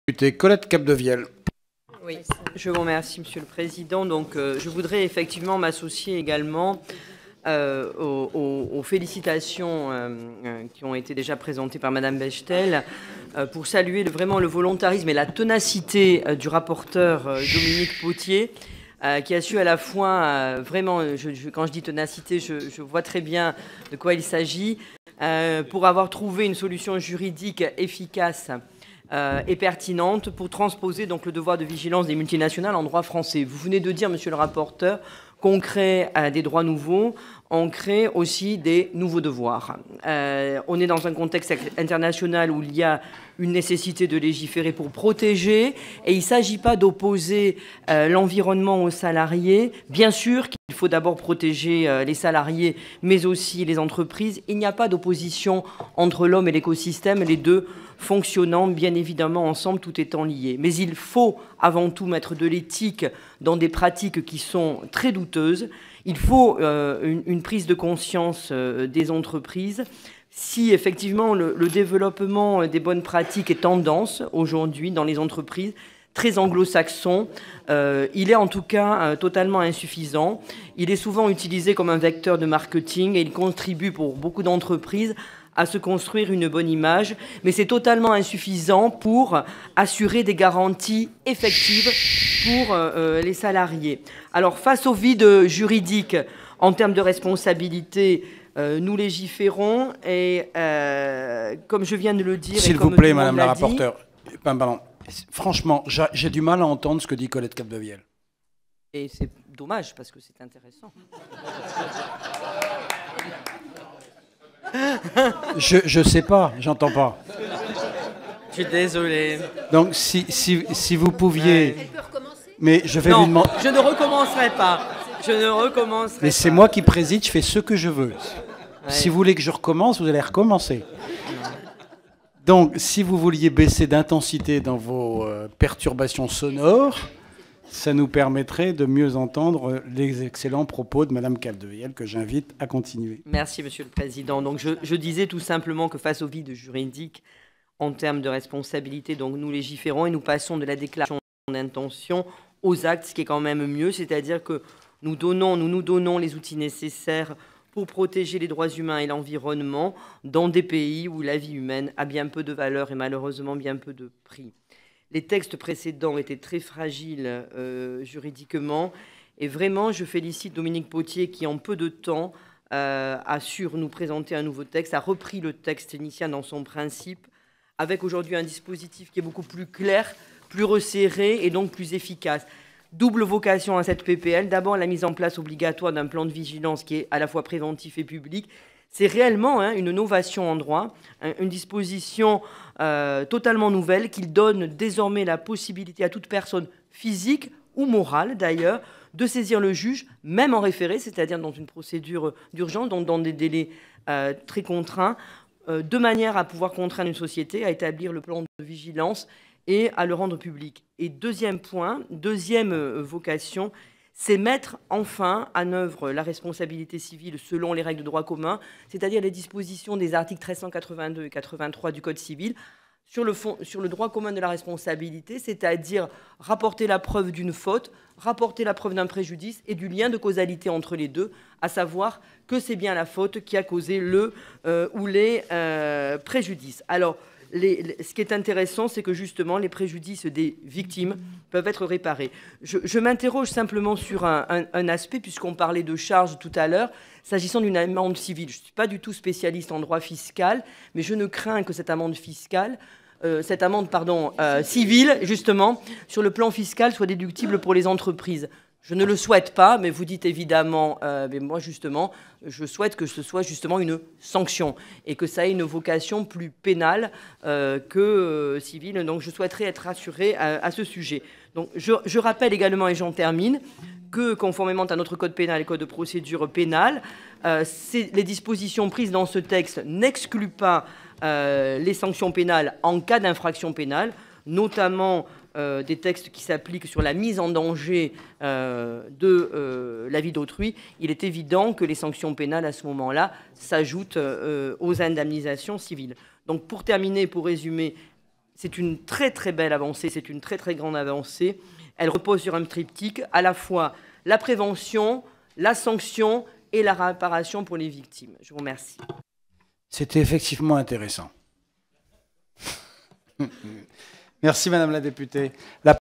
"Sociétés mères et entreprises donneuses d'ordre" (PPL)
Ce matin (11 mars 2015) en Commission des lois, je suis intervenue pour défendre la Proposition de loi relative au "devoir de vigilance des sociétés mères et entreprises donneuses d'ordre".

La France est le premier pays à se doter d'une législation aussi novatrice et volontariste.